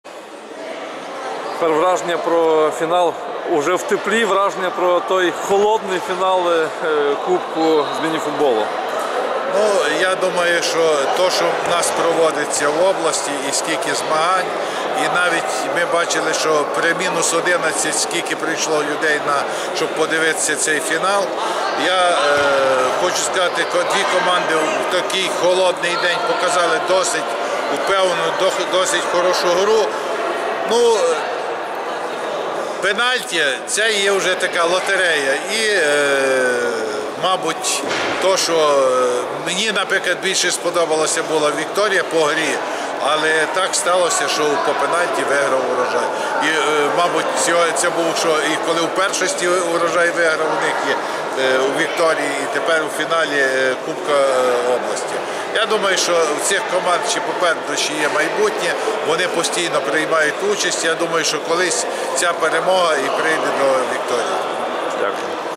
Кубок області 2018 фінал (Василь Орлецький)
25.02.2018 р. Майданчик стадіону "Буковина".
Кубок Чернівецької області з міні-футболу, фінал
Урожай - Вікторія-Везувій
Відео: fakty.cv.ua (партнер - СК "Гешко")